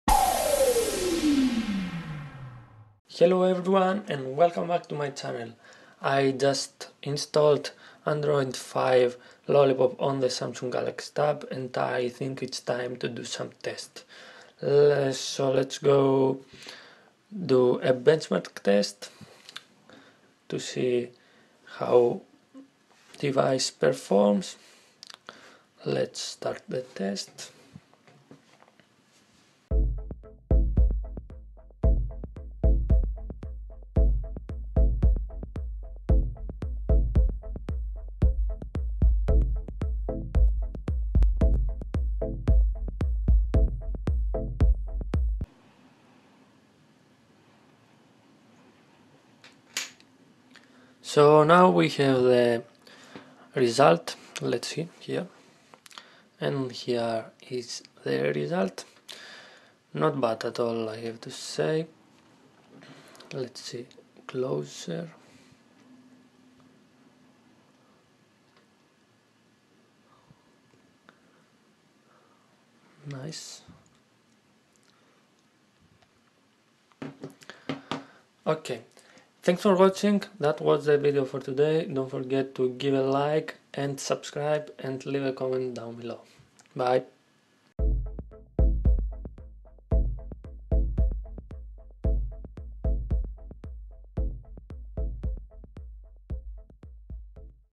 Samsung galaxy tab with android 5 lollipop|BENCHMARK TEST
This is a benchmark test on the samsung galaxy tab gtp1000 running android 5 lollipop.Hope to enjoy!

( OMNI ROM android 4.4.1 kitkat|BENCHMARK TEST)

(INSTALL android 5 lollipop on galaxy tab)